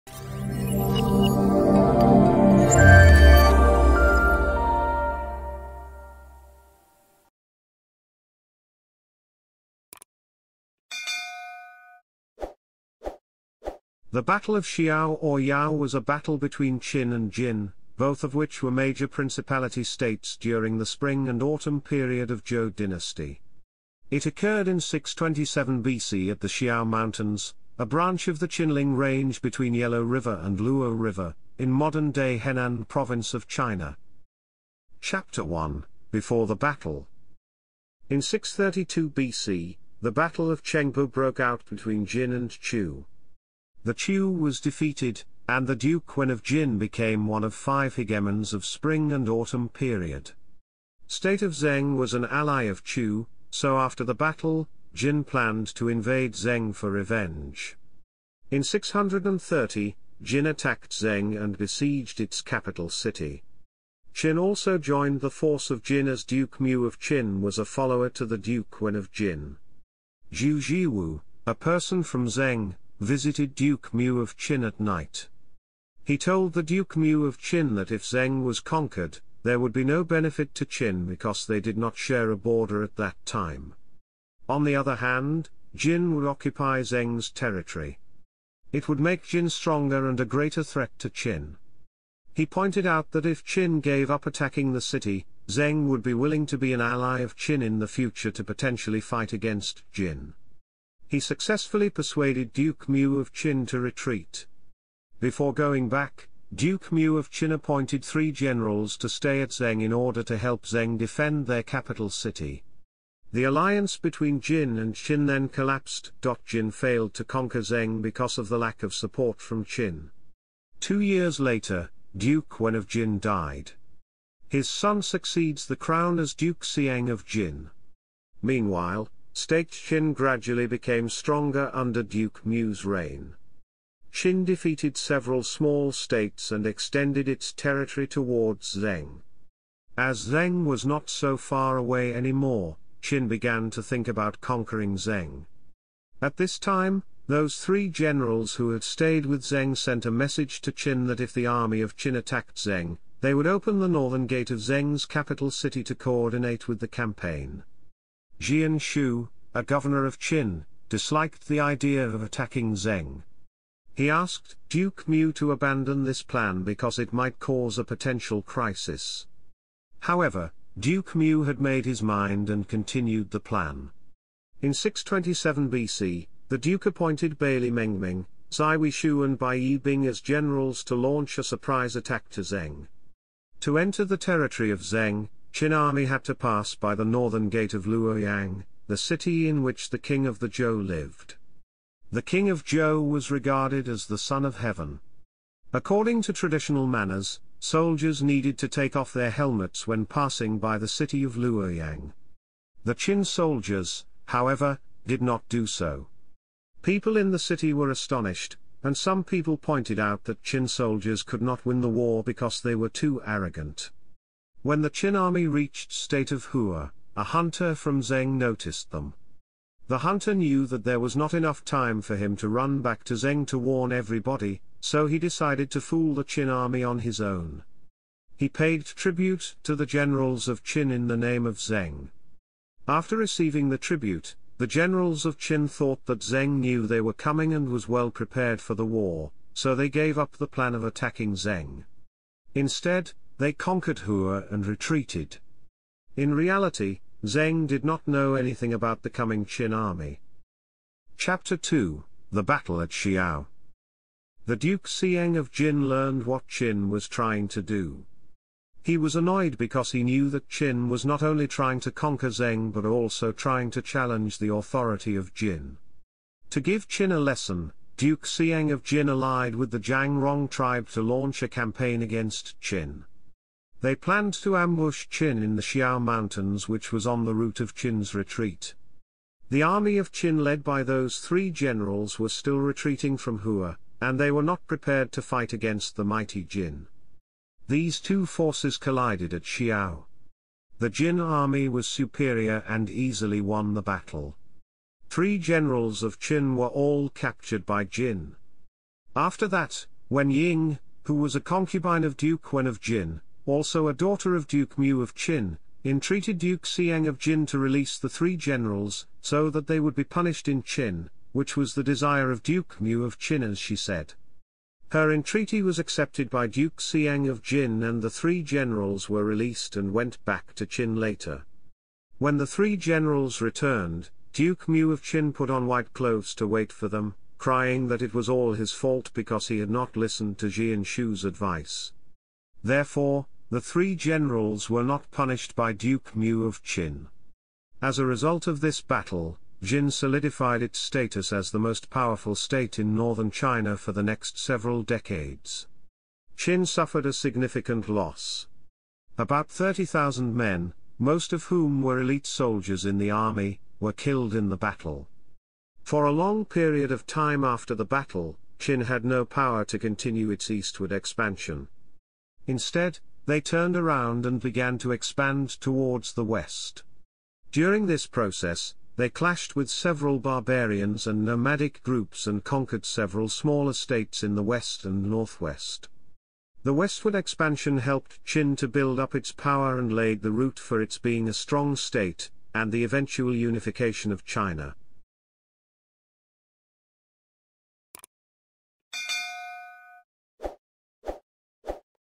Battle of Xiao - Wikipedia article
Video summary:
The Battle of Xiao or Yao (Chinese: 殽之戰) was a battle between Qin and Jin, both of which were major principality states during the Spring and Autumn Period of Zhou dynasty. It occurred in 627 BC at the Xiao Mountains, a branch of the Qinling Range between Yellow River and Luo River, in modern-day Henan province of China.